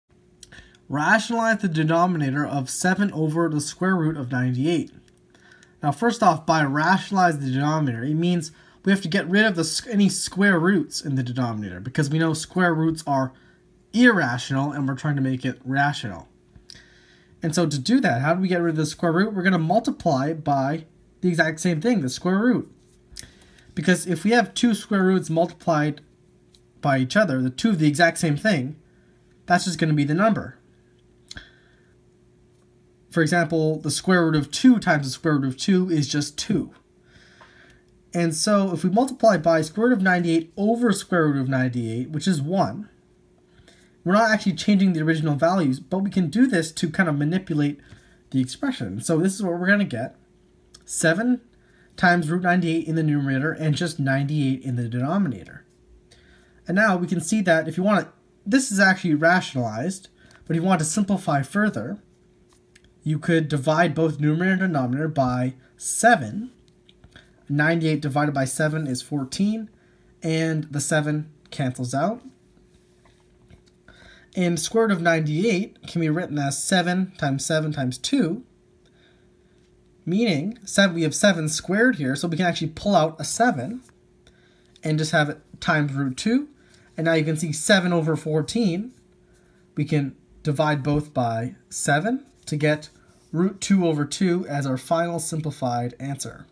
Rationalizing a Monomial Denominator (Example)
Rationalize the denominator of 7/sqrt(98)

Need a math tutor? I can help you!
~ For more quick examples, check out the other videos on my youtube channel